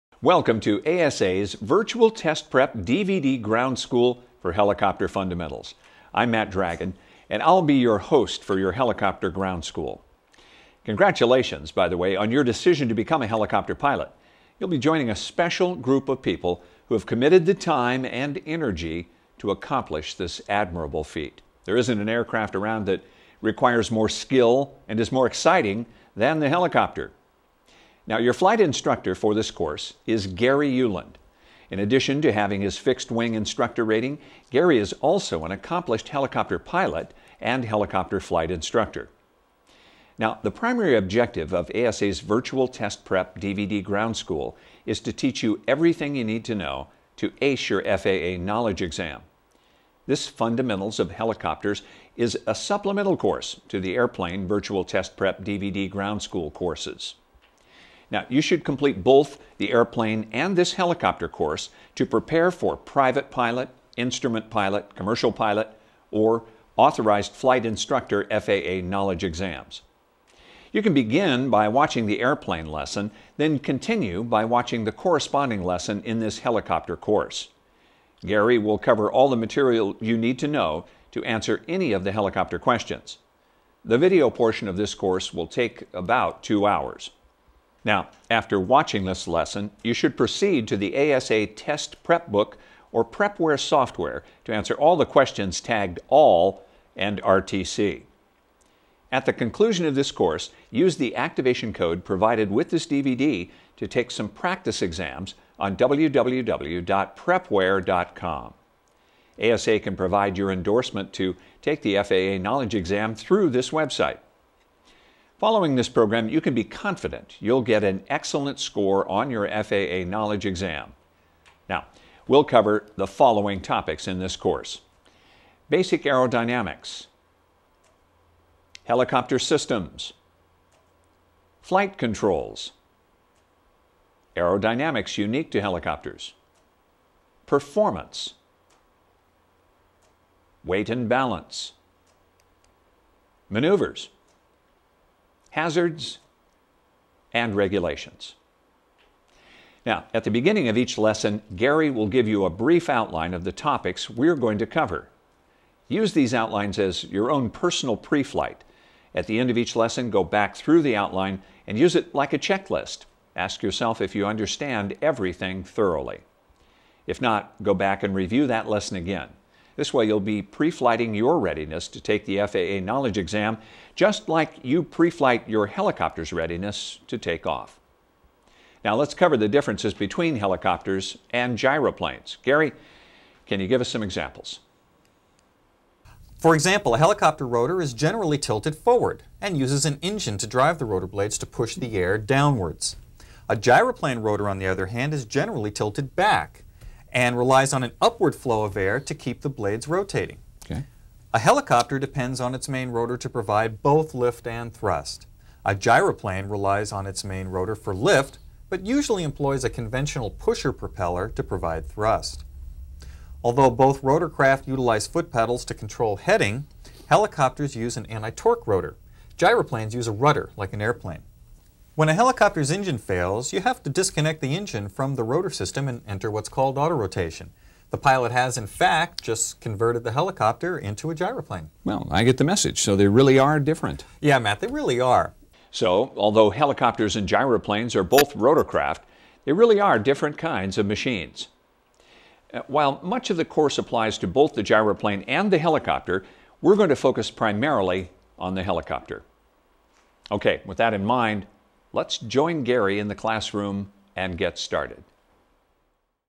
Helicopter - Introduction - ASA (Aviation Supplies & Academics)
From the Helicopter Fundamentals Virtual Test Prep DVD ground school, this video is an introduction to the content found int he course and video segment downloads.